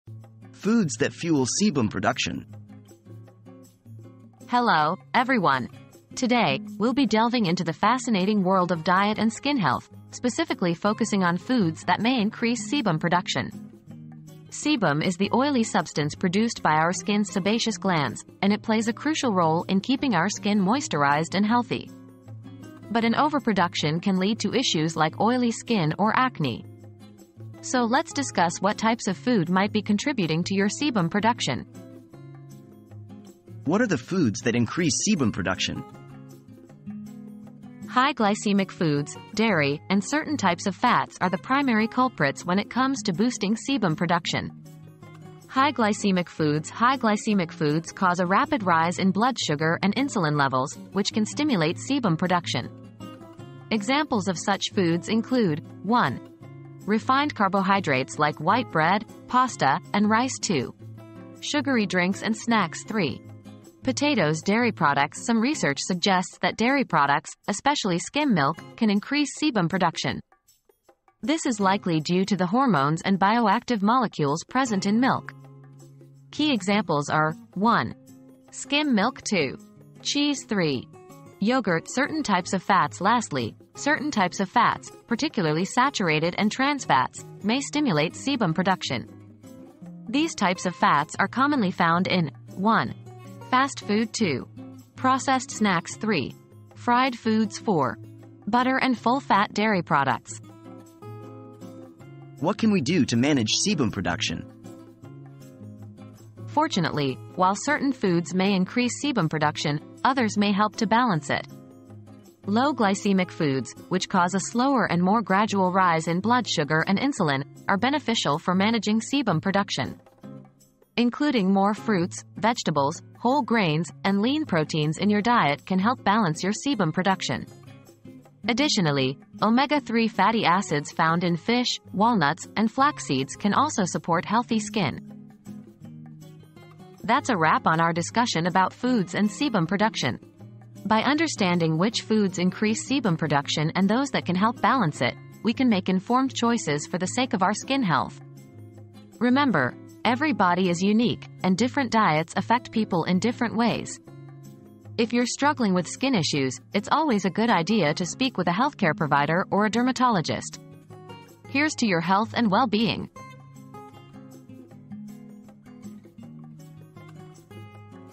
Foods That Fuel Sebum Production
What are some examples of foods that produce sebum? • Discover which foods may be contributing to your oily skin or acne in this informative video on foods that fuel sebum production. Learn about high glycemic foods, dairy products, and certain types of fats that may stimulate sebum production, as well as low glycemic foods and omega-3 fatty acids that can help balance it. Don't miss out on this essential guide to diet and skin health!

00:00 • Foods That Fuel Sebum Production
00:33 • What are the foods that increase sebum production?
01:42 • What can we do to manage sebum production?